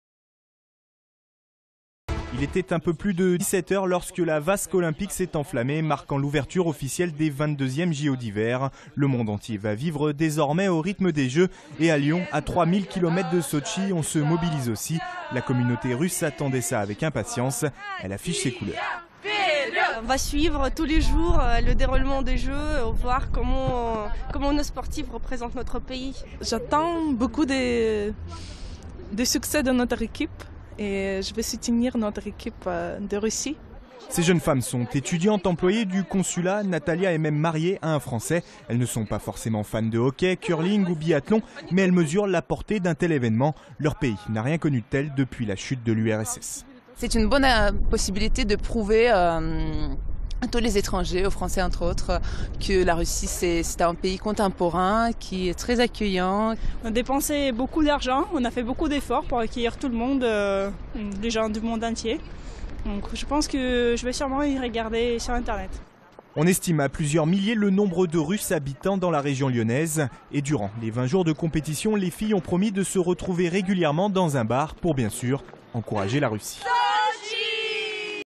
Coup d'envoi des JO de Sotchi
Sites webs/blogs, copiez cette vidéo avec le bouton "Intégrer" !
 La communauté russe de Lyon se mobilisent pour les Jeux Olympiques de Sotchi.